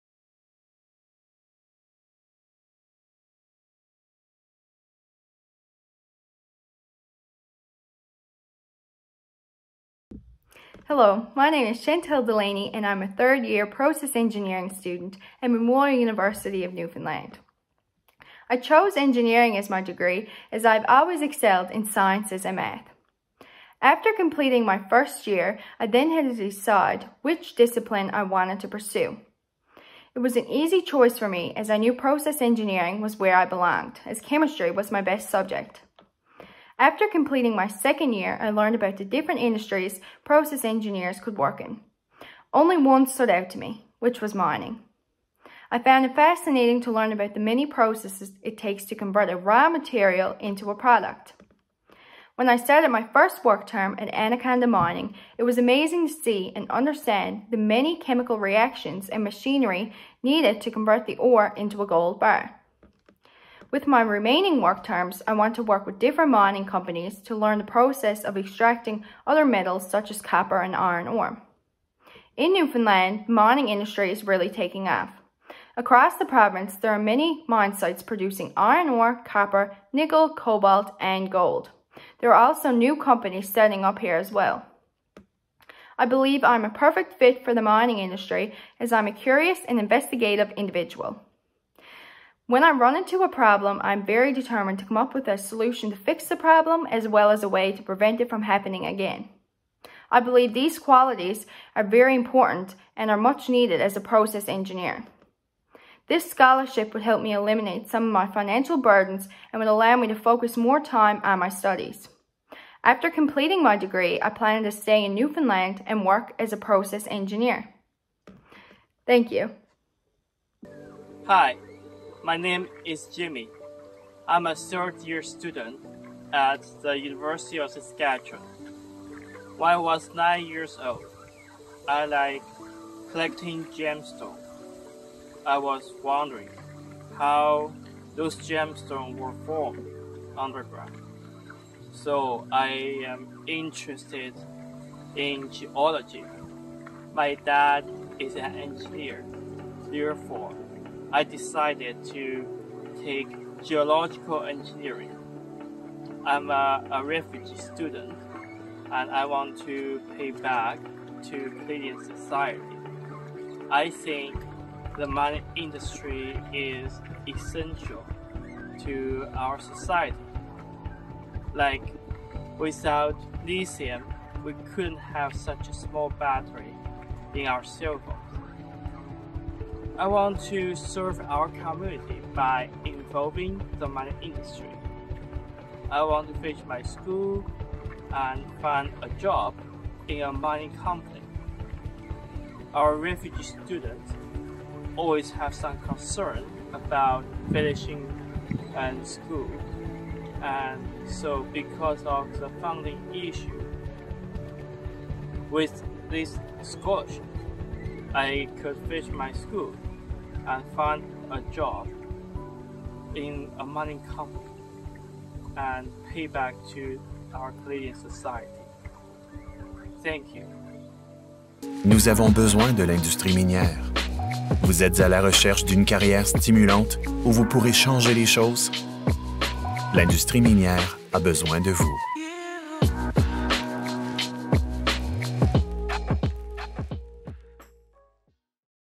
J’ai choisi l’industrie minière. L’industrie minière m’a choisi - bourses d’études gagnantes 2021
En savoir plus sur Chantelle et Jimmy, les gagnantes de bourses d’études sur le 25e anniversaire RHiM.